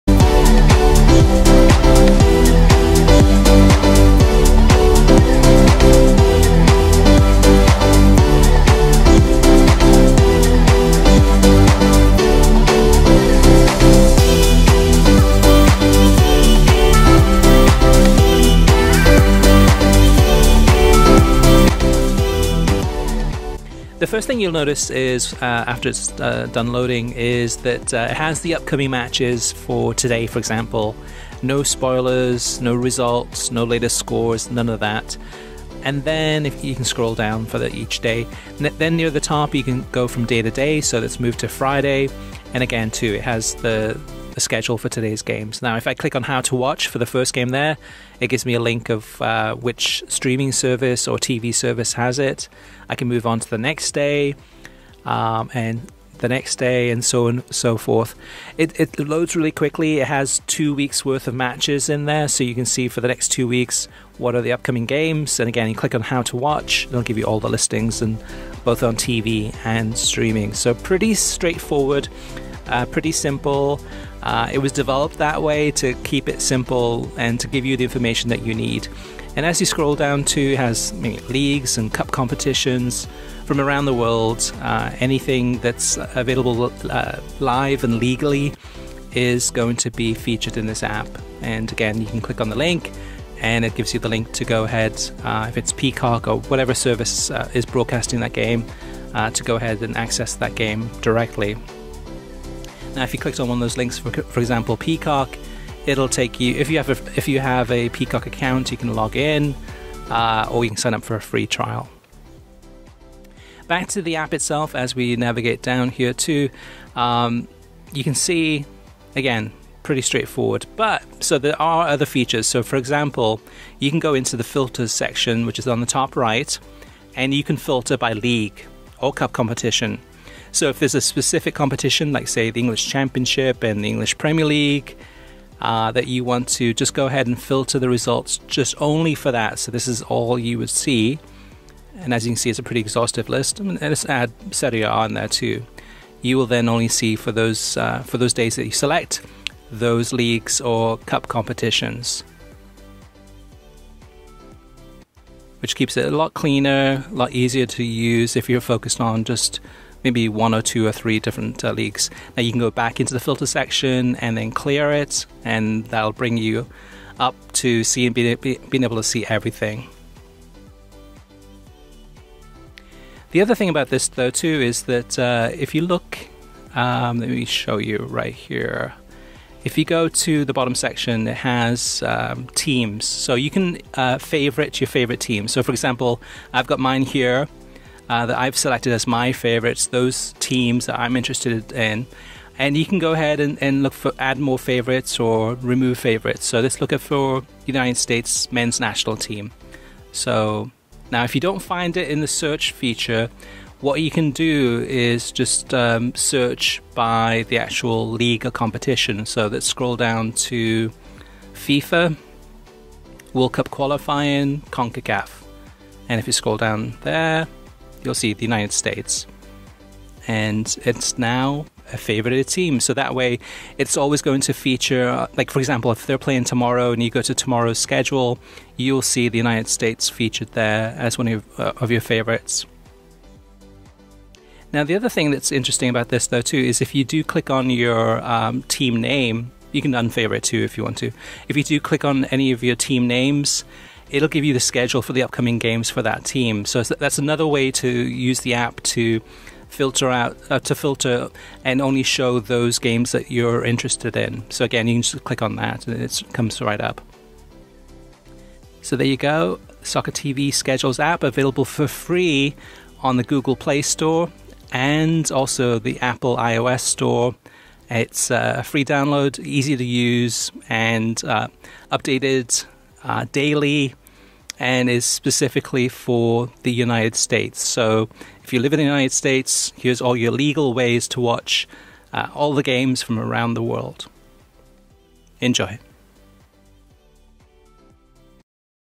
Soccer TV Schedules App: Brief Demo
Watch a brief demo of the Soccer TV Schedules App to find out the tips and tricks to get the most out of using it. Don't miss a single game of your favorite team with this helpful soccer TV schedules app.